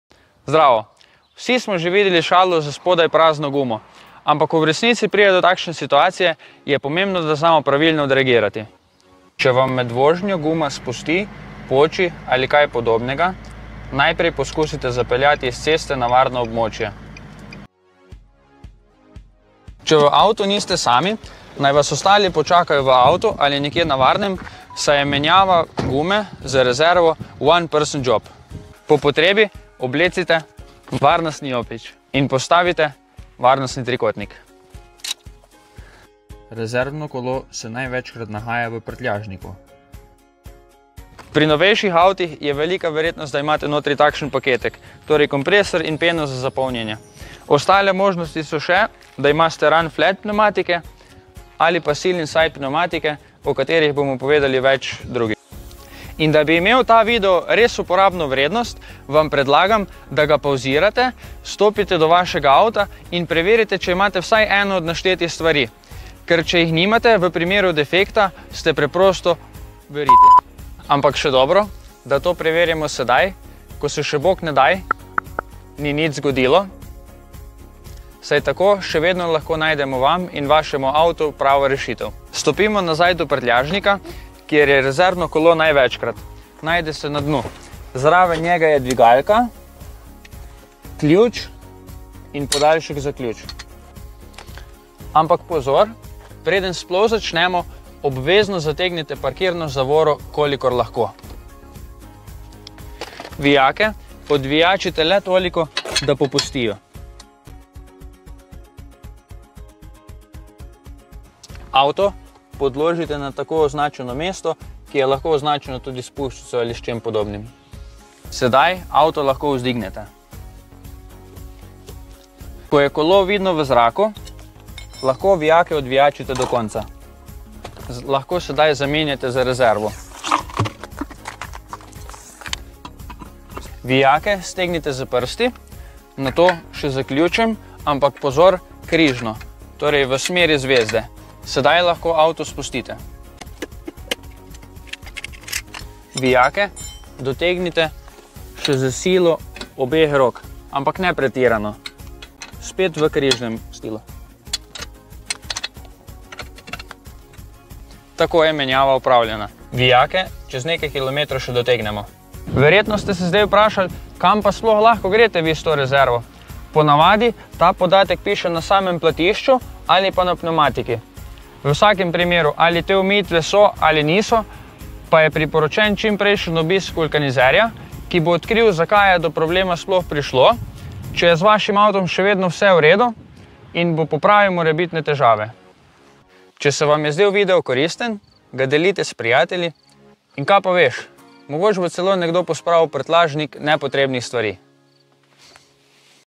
Kaj storiti ob gumidefektu?
Upajmo, da v to situacijo ne pridete, a če pridete upamo, da vam bo ta video pomagal.
-počena guma, rezervno kolo, kako zamenjati gumo, pukla guma, kako promijeniti gumu, how to change a tire, flat tire, prazna guma, rezervno kolo